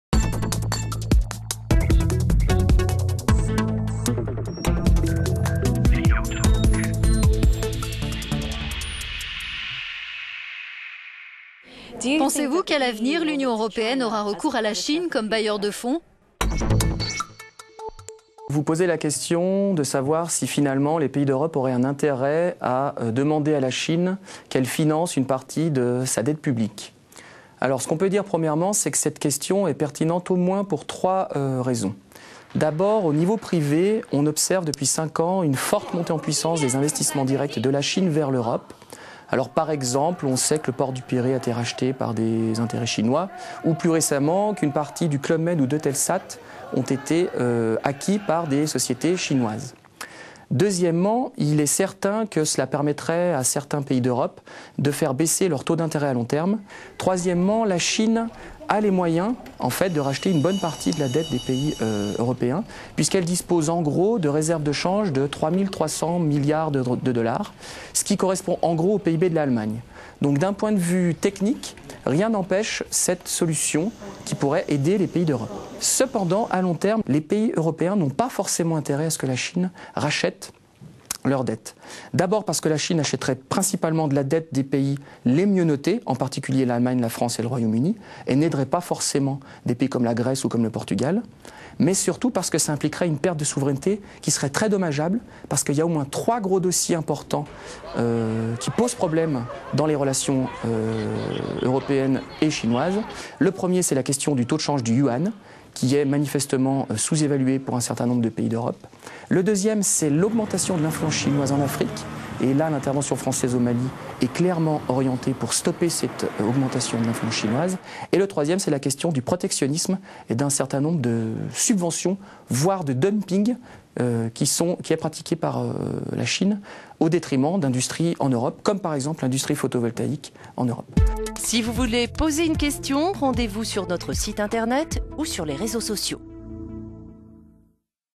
Faut-il laisser la Chine investir en Europe ? - utalk
Nicola, Londres :
"Pensez-vous qu'à l'avenir, l'Union européenne aura recours à la Chine comme bailleur de fonds ?"

La réponse de Grégory Vanel, économiste et politologue :
"Vous posez la question de savoir si les pays d'Europe auraient un intérêt à demander à la Chine de financer une partie de leur dette publique. Sachez que cette question est pertinente pour au moins trois raisons : d'abord au niveau privé, on observe depuis cinq ans, une forte montée en puissance des investissements directs de la Chine vers l'Europe. Par exemple, on sait que le port du Pirée a été racheté par des intérêts chinois ou plus récemment qu'une partie du Club Méd et d'Eutelsat ont été acquises par des sociétés chinoises. Deuxièmement, il est certain que cela permettrait à certains pays d'Europe de faire baisser leurs taux d'intérêt à long terme et enfin, la Chine a les moyens de racheter une bonne partie de la dette des pays européens puisqu'elle dispose de réserves de change pour environ 3300 milliards de dollars, ce qui correspond en gros, au PIB de l'Allemagne.

Donc d'un point de vue technique, rien n'empêche cette solution qui pourrait aider les pays d'Europe. Cependant à long terme, les pays européens n'ont pas forcément intérêt à ce que la Chine rachète leurs dettes : d'abord parce que la Chine achèterait principalement de la dette des pays les mieux notés, en particulier l'Allemagne, la France et le Royaume-Uni et n'aiderait pas nécessairement des pays comme la Grèce ou le Portugal, mais surtout parce que cela impliquerait une perte de souveraineté très dommageable pour les Etats européens.

Au moins trois gros dossiers importants posent problème dans les relations européennes et chinoises : le premier, c'est la question du taux de change du yuan qui est manifestement sous-évalué pour un certain nombre de pays d'Europe. Le deuxième, c'est l'augmentation de l'influence chinoise en Afrique et là, l'intervention française au Mali est clairement orientée pour stopper cette augmentation de l'influence chinoise et le troisième problème, c'est la question du protectionnisme et d'un certain nombre de subventions, voire de dumping qui est pratiqué par la Chine au détriment d'industries en Europe, comme par exemple, l'industrie photovoltaïque en Europe."